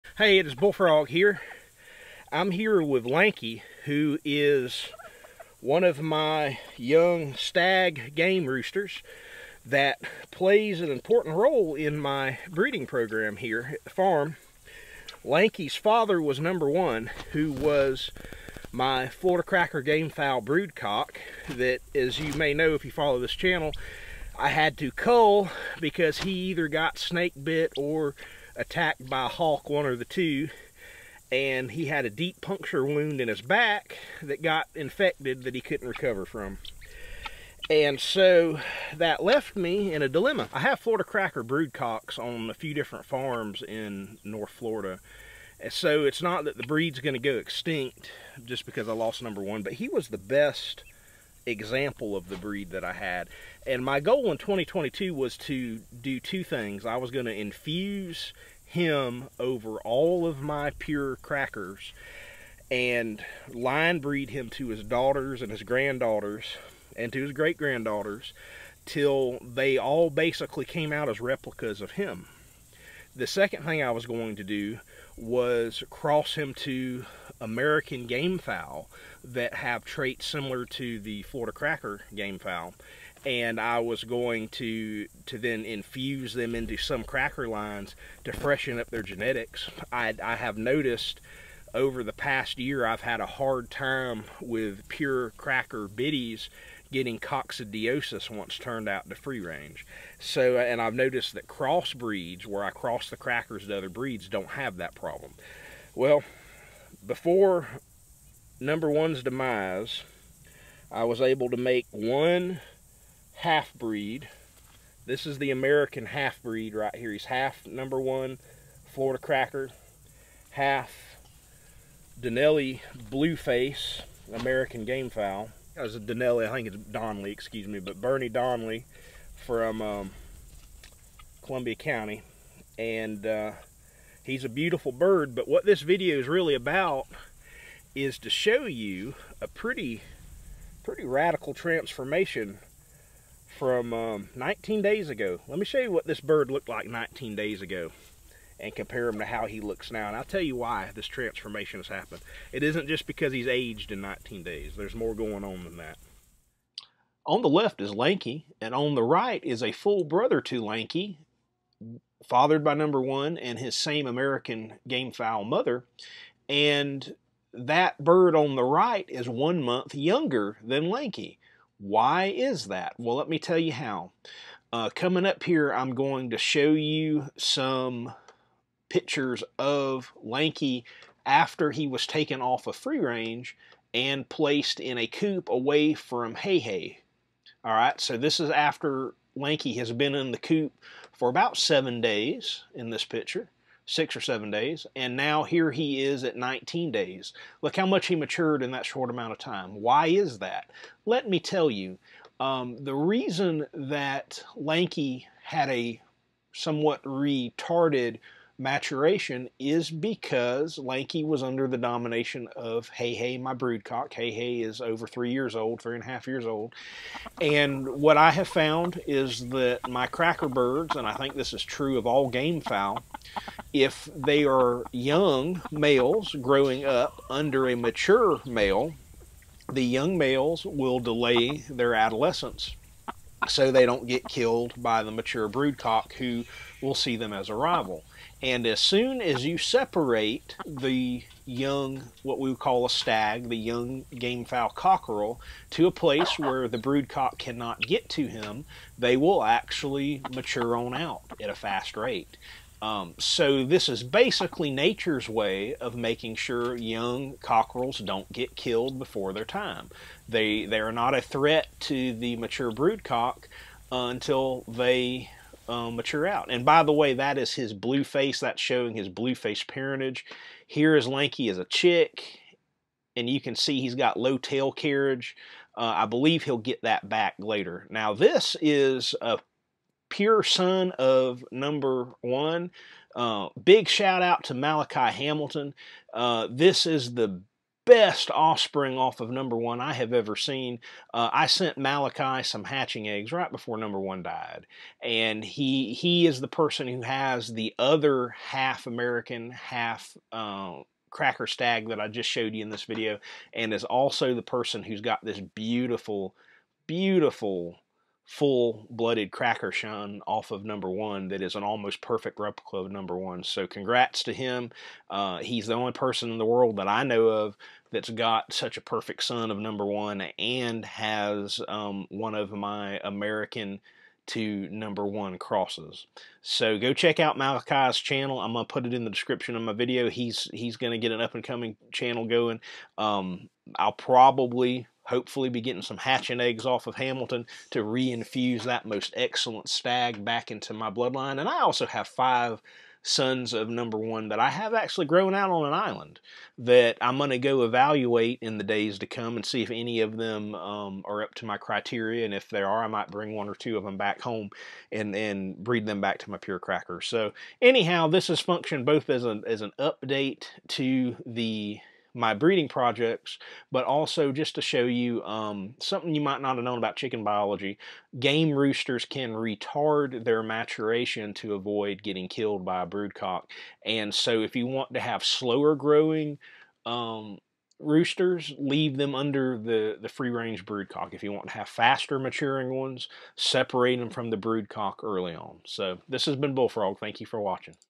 This One Trick Will Make Your Roosters Mature Fast!
Here’s how you can speed up or slow down the maturation process of a rooster.